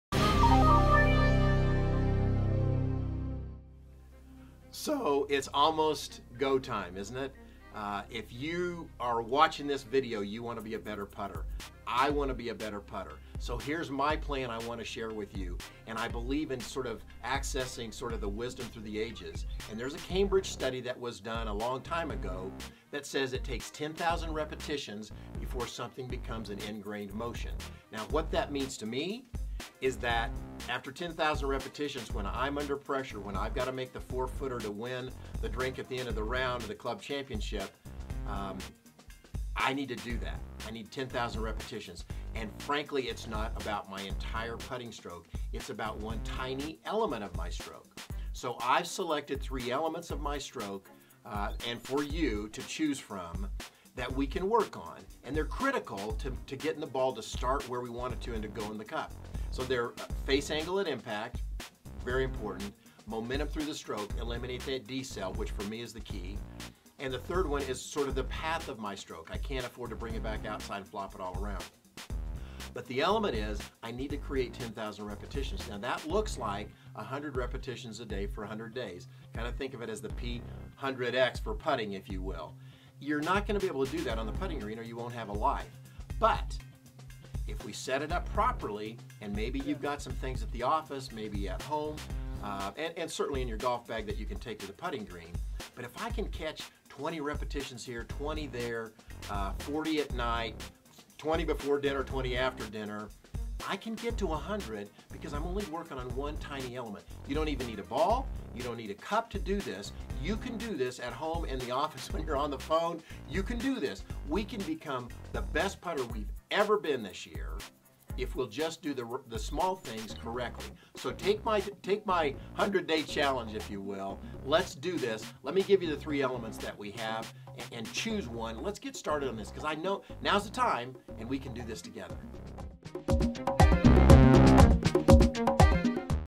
10,000 Stroke Challenge by EyeLine Golf
The Cambridge Study states that it take 10,000 repetitions for a movement to become engrained.
Let's build a pressure proof stroke!
Select a single element of your stroke and make 10,000 repetitions.
Take the 100 rep per day, 100 day challenge to great putting :)